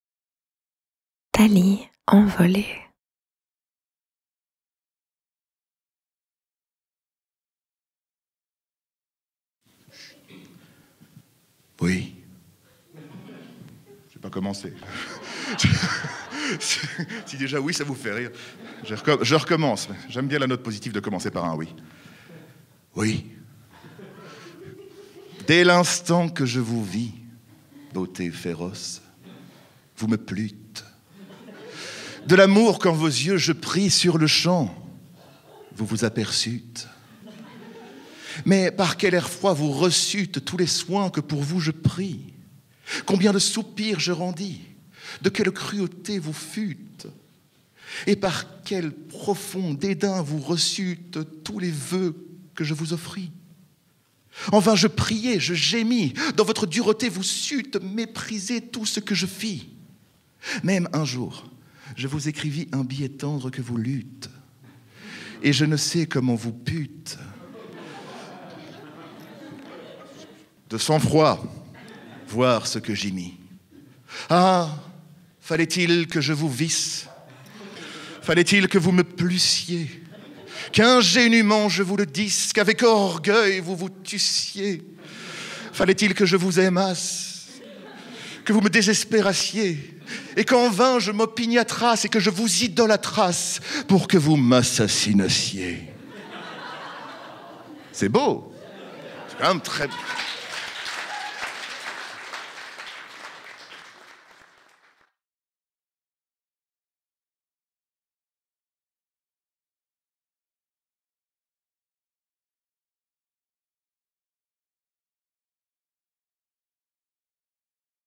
Complainte amoureuse - Alphonse Allais - LIVE - Thalie Envolée (HD)
Enregistrement live du texte « Complainte amoureuse » attribué à Alphonse Allais lors du spectacle « Poésie Comedy Club » le 10 octobre 2020 au Centre Culturel Bruegel, à Bruxelles, dans le cadre du projet Thalie Envolée

Crédits :
Texte : (attribué à) Alphonse Allais
Interprétation : Maxime Anselin
Mise en scène : Maxime Anselin & Julie Verleye
Mixage : Matthieu Van Dyck
Captation & montage : Antoine Motte dit Falisse.